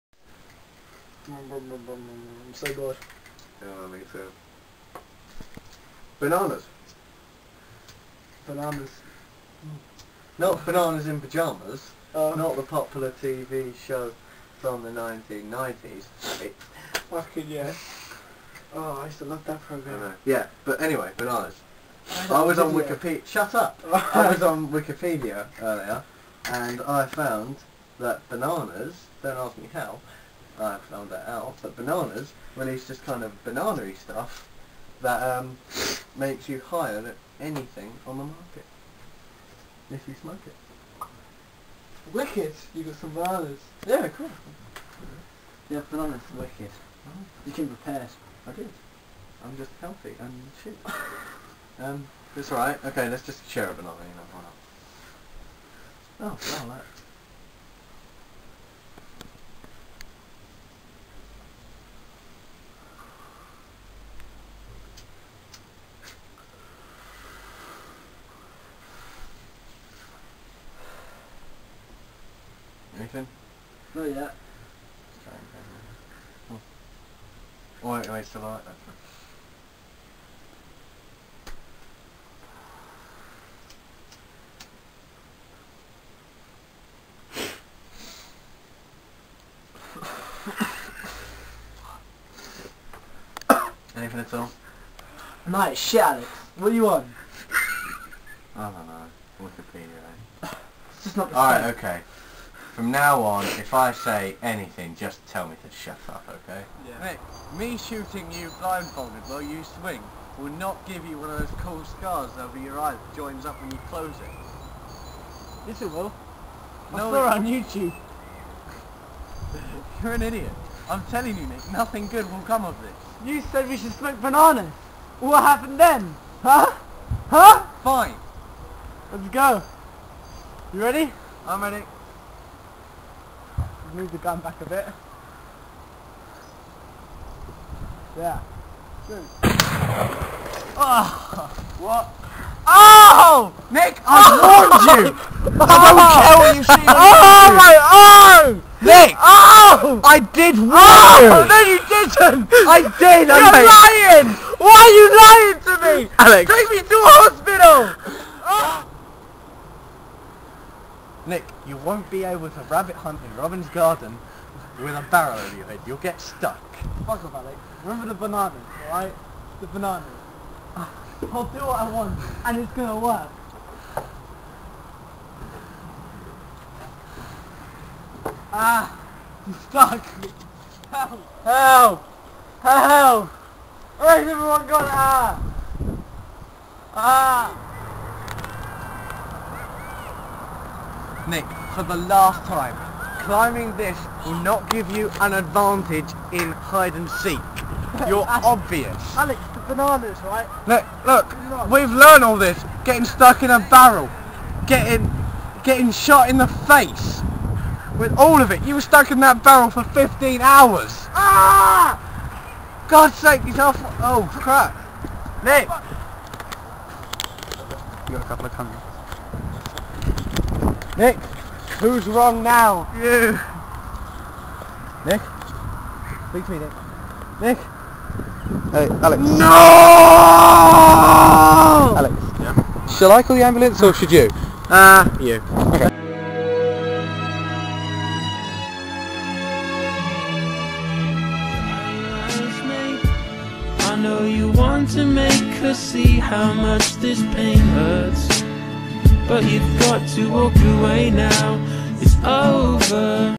banana smoking
what starts off as harmless time-wasting on a boring day soon turns into justified stupidity for nick and 6 weeks stress leave for alex after filming